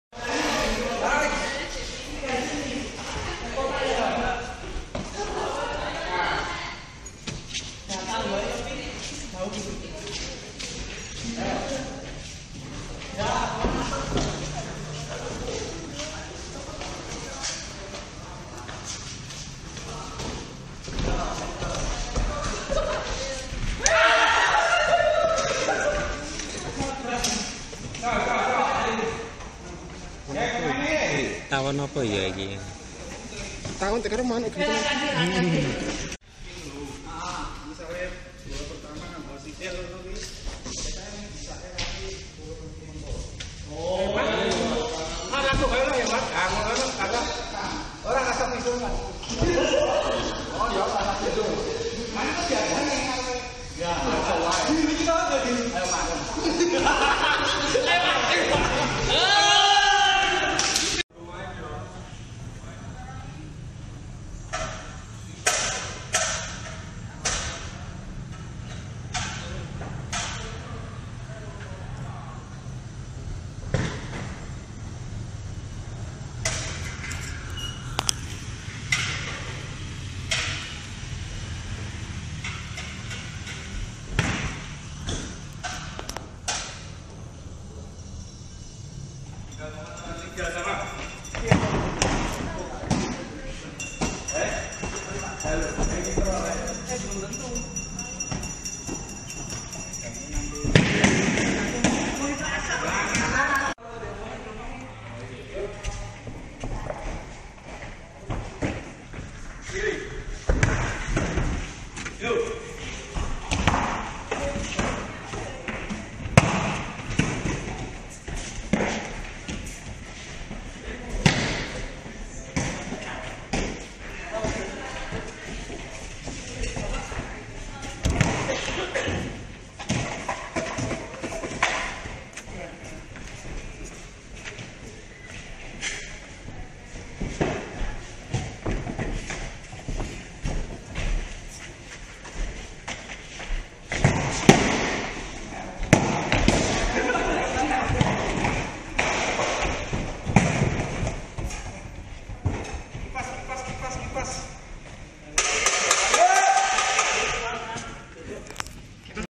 Tawon hitam dan besar sedang mencari makan di dalam bunga
Ada  tawon yang sedang hinggap di bunga, tawon dengan warna hitam dan besar ini sedang mencari makan. Sangat besar dan cukup ngeri bila menyengat.